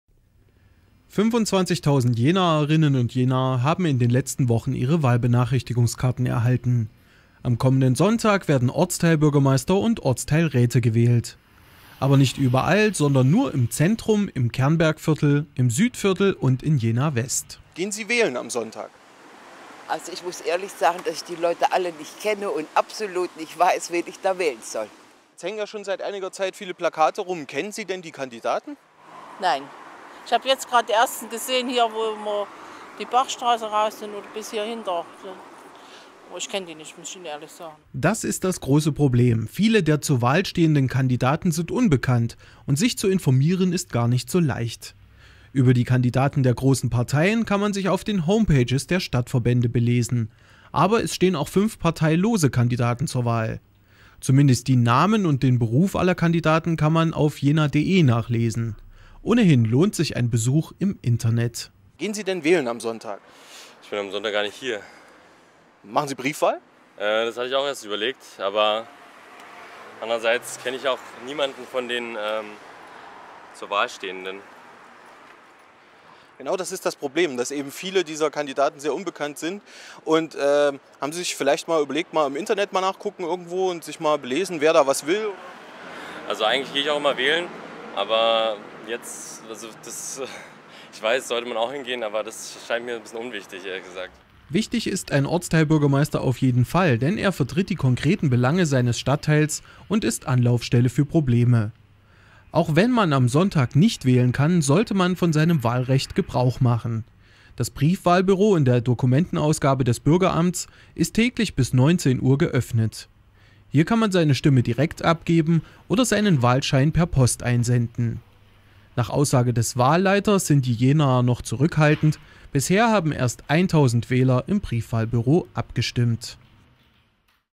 Premiere: In den letzten vier Ortsteilen werden Bürgermeister gewählt
Der kommende Sonntag ist Wahltag. Aber nicht für alle Jenaer, sondern nur für die Bewohner des Zentrums, der Kernberge, des West- und des Südviertels. Diese Stadtteile sind die letzten vier, die  einen Ortsteilbürgermeister und einen Ortsteilrat bekommen sollen. Was die Bürger davon halten -- wir haben heute nachgefragt.